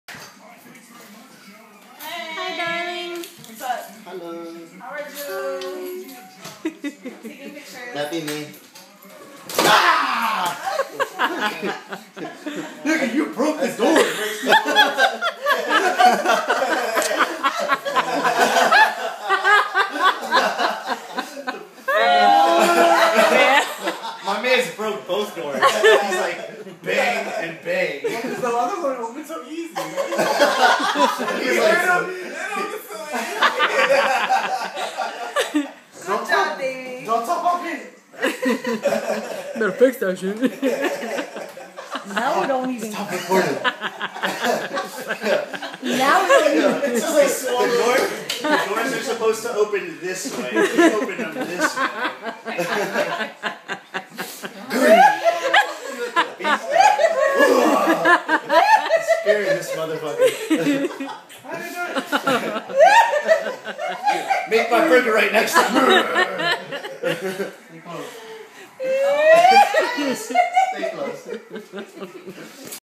Scare Prank Gone Wrong
We're at my cousin Jay's house, and my cuz Dave thinks it'll be funny to scare him as he walks in the door from work. Watch and learn... Firm footing! Lol ok that's not relevant but whatever lol. Enjoy!